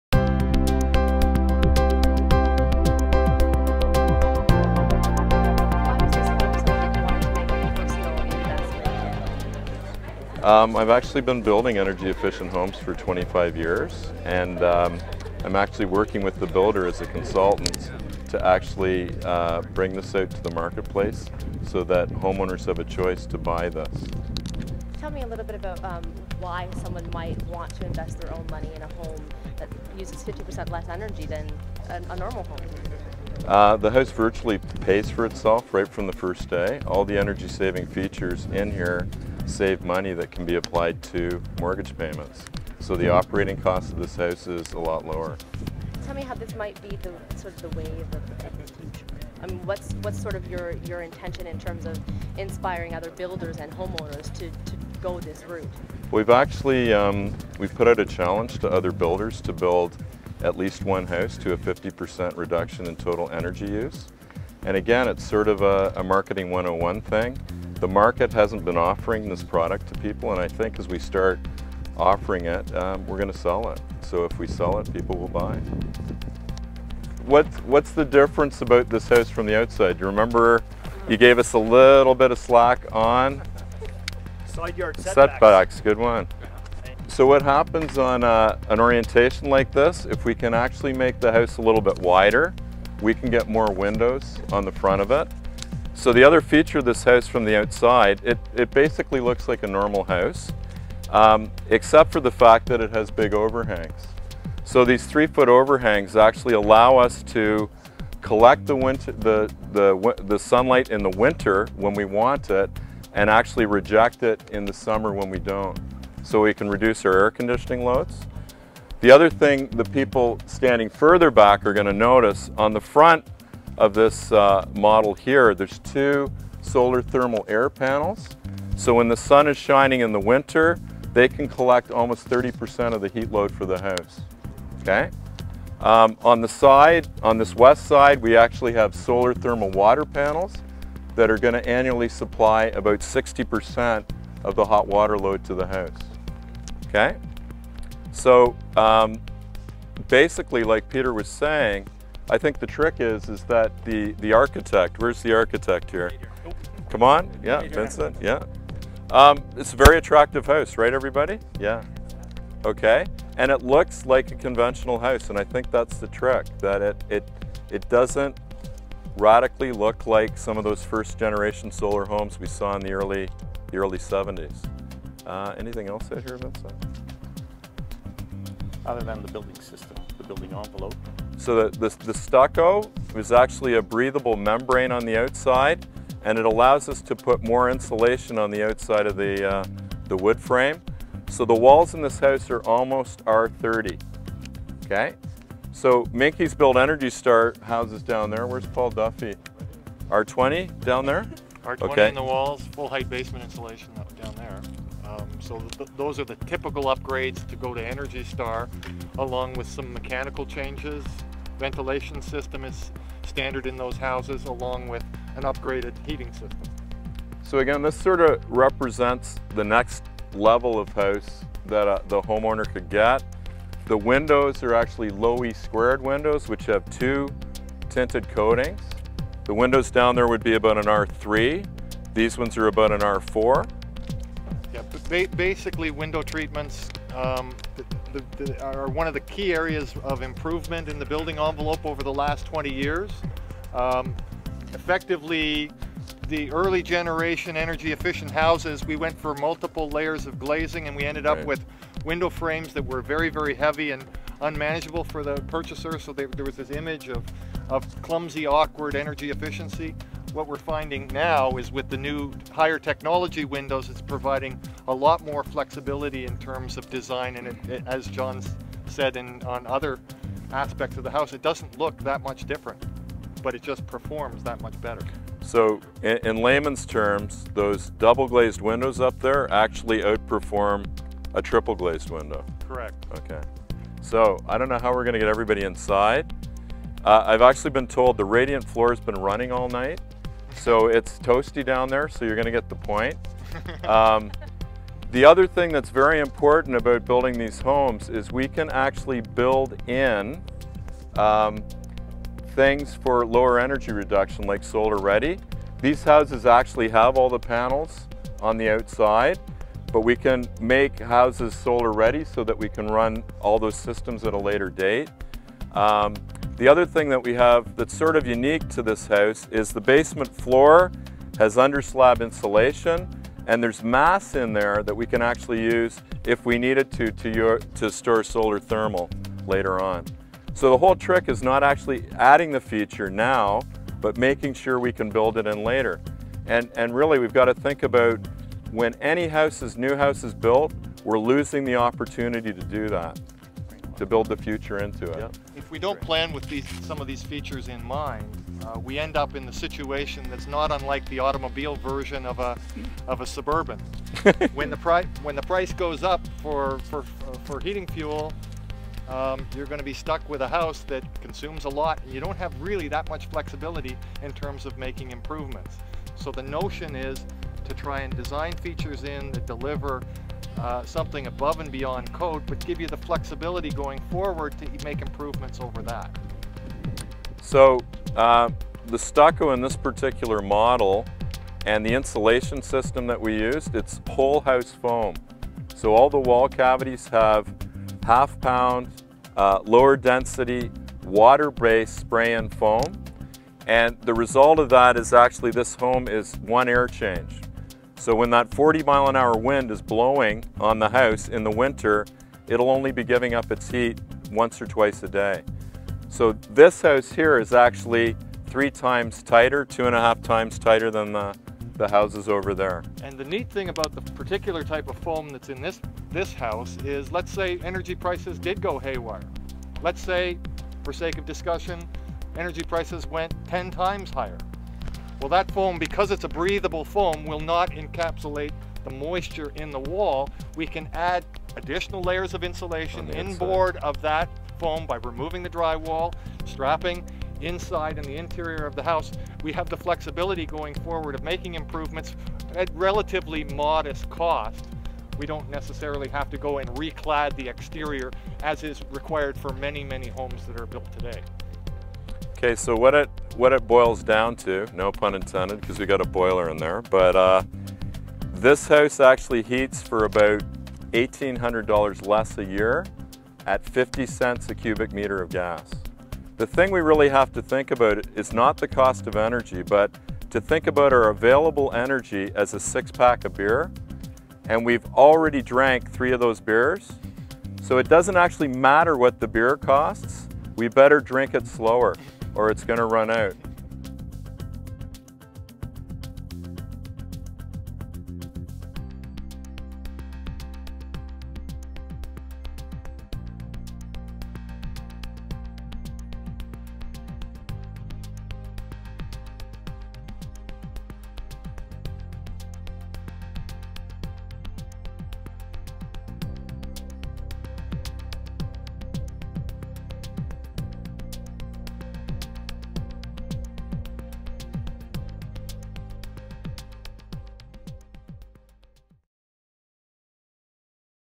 GreenHomeTV - John Godden Paul Duffy - Part 1 - Tour of Newmarket EcoLogic Homes by Rodeo Homes
Join John Godden of ClearSphere, John Duffy of Icynene and visiting dignitaries on a tour of Rodeo Fine Homes EcoLogic Home in Newmarket, Ontario, Canada.  These homes are the first LEED Platinum community of 34 homes.  This is part 1 of a 3 part series.  This segment features the outside structural features that contribute to the homes LEED rating.